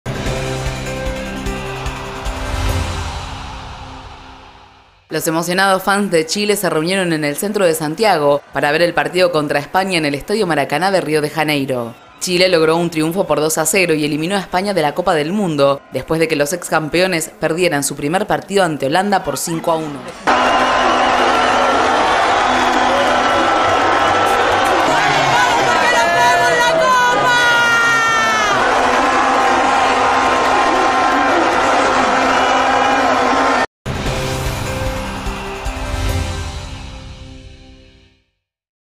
Cientos de chilenos festejaron en las calles de Santiago el triunfo ante España
Cientos de chilenos se reunieron en el centro de Santiago para ver el partido que su seleccionado de fútbol disputó ante España en el estadio Maracaná de Río de Janeiro.

Chile logró un triunfo por 2-0 y eliminó a España de la Copa del Mundo después de que los ex campeones perdieron su primer partido ante Holanda por 5-1.